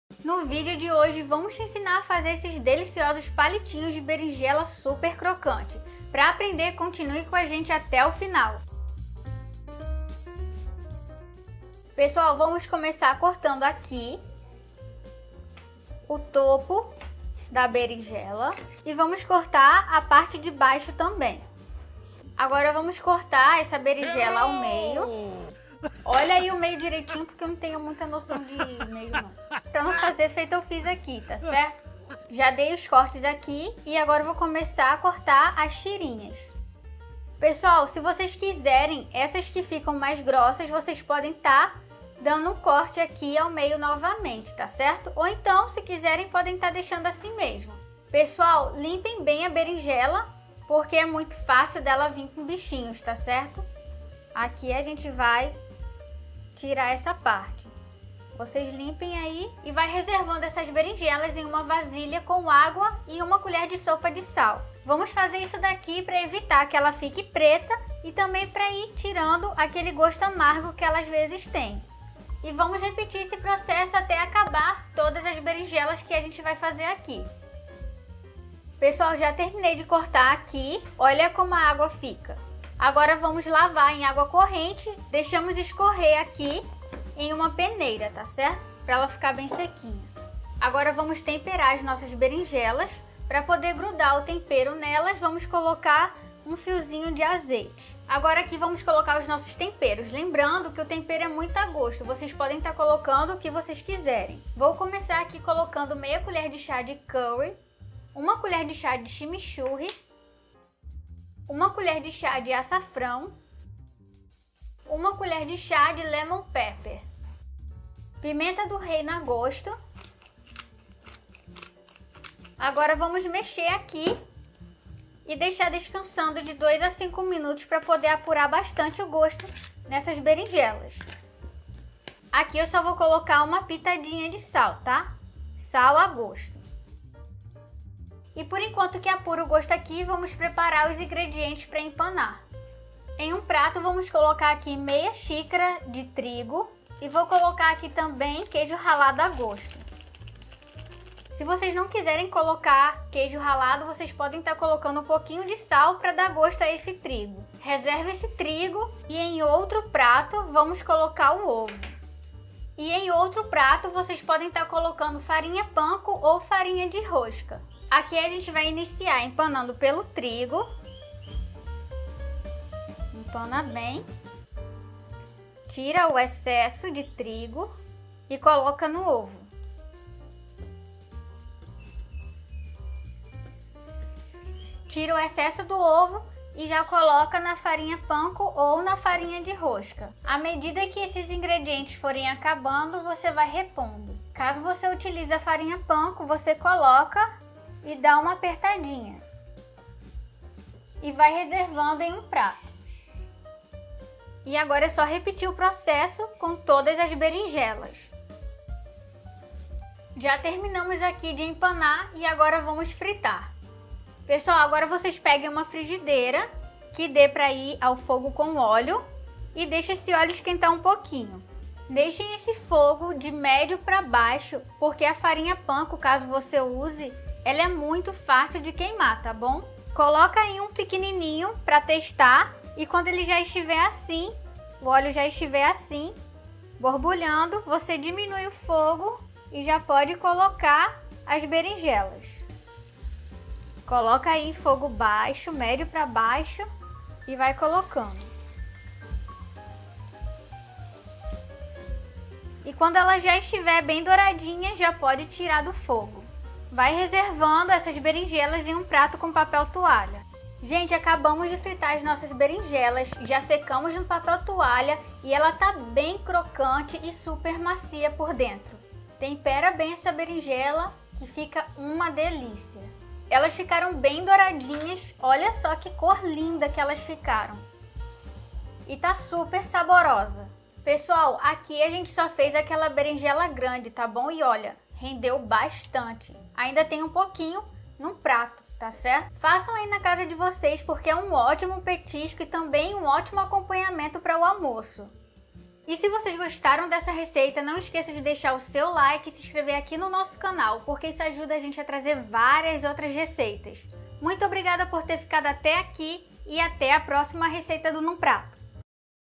PALITINHOS DE BERINJELA | Berinjela empanada crocante
Hoje a gente vai te ensinar a fazer palitos de berinjela super crocantes e sequinhos, uma berinjela temperada empanada em tirinhas deliciosas! Um acompanhamento ideal para o seu almoço.

CURSOS E EBOOKS QUE RECOMENDAMOS:
- Livre-se da fritura com mais de 150 receitas deliciosas e saudáveis na airfryer

INGREDIENTES:
- 1 berinjela grande;
- azeite a gosto;
- 1/2 colher de chá de curry;
- 1 colher de chá de açafrão;
- 1 colher de chá de chimichurri;
- 1 colher de chá de lemon pepper;
- pimenta do reino a gosto;
- 1/2 xíc. de trigo;
- queijo ralado a gosto;
- 1 ovo;
- 1/2 xíc. de farinha panko ou de rosca;

MODO DE PREPARO:
- Para pegar todas as dicas e não ter erro na sua receita, assista ao vídeo até o final.

Ah, e se fizer a receita marca a gente lá no Instagram que vamos comentar, combinado?

Na receita ensinamos como fazer berinjela empanada crocante e sequinha, um prato perfeito para servir de acompanhamento para o seu almoço ou jantar.
O vídeo tá recheado de dicas, ensinamos como tirar o amargo da berinjela e muito mais, tudo de maneira rápida e fácil, perfeito pra te ajudar a fazer um almoço simples e rápido.
Já separa aí os ingredientes e vem fazer essa beringela frita crocante, e sim, você também pode fazer na airfryer! Bora cozinhar?